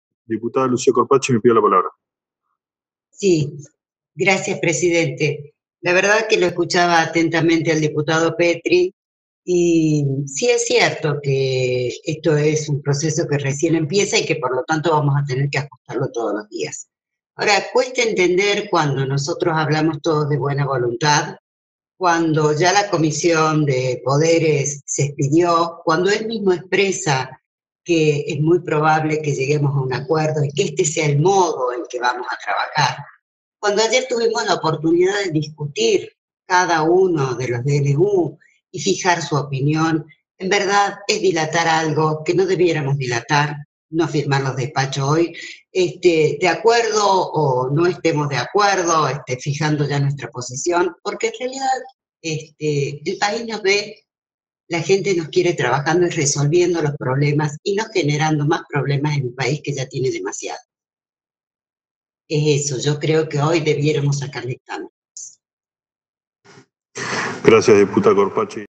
Diputada Corpacci, Lucía - C. Bicameral Permanente de Trámite Legislativo - 29-04-2020
VIDEOCONFERENCIA: Comisión Bicameral Permanente de Trámite Legislativo - DNU.